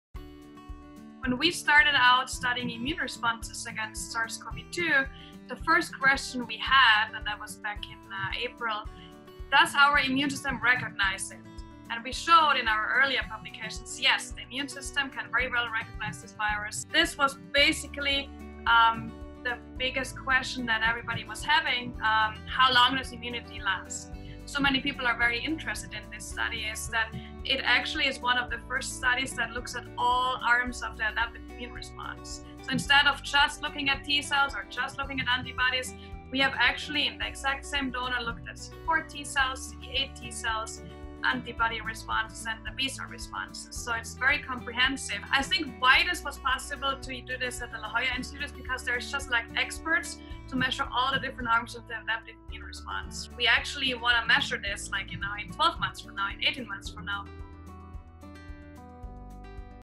Protective immunity against SARS-CoV-2 could last eight months or more
New research demonstrates that declining antibodies don't spell disaster for long-lasting immunity against SARS-CoV-2.